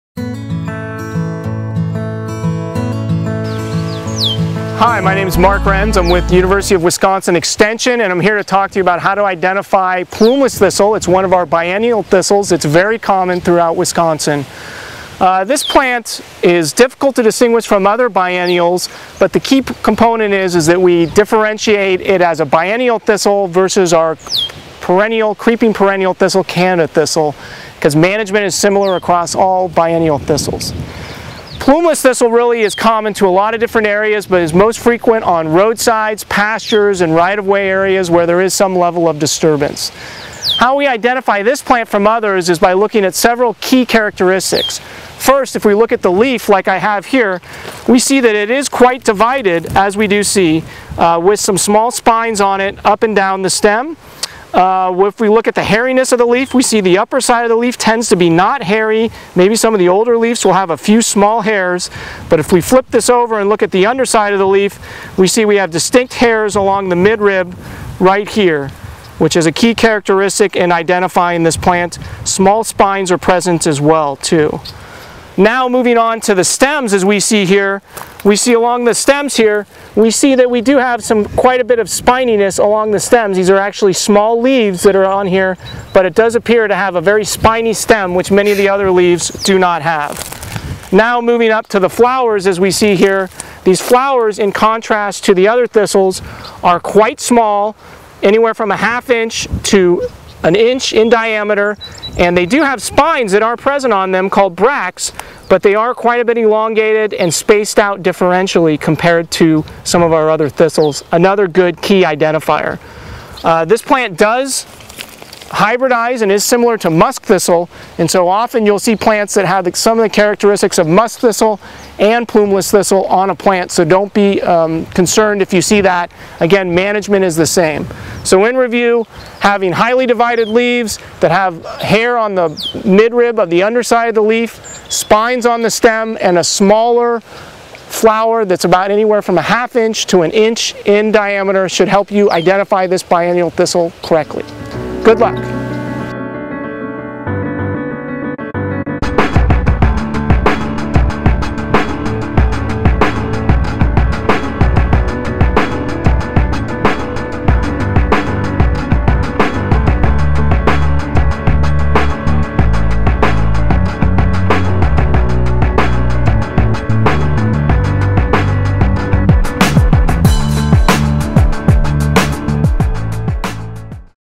Plumeless Thistle, identification of the Wisconsin Invasive Species Carduus acanthoides
This is part of a series of videos providing key characteristics for the identification of invasive plants listed in Wisconsin's invasive species administrative rule NR 40. These videos are produced by Dr. Mark Renz of the University of Wisconsin-Madison. For more information on invasive plants and invasive plant management in Wisconsin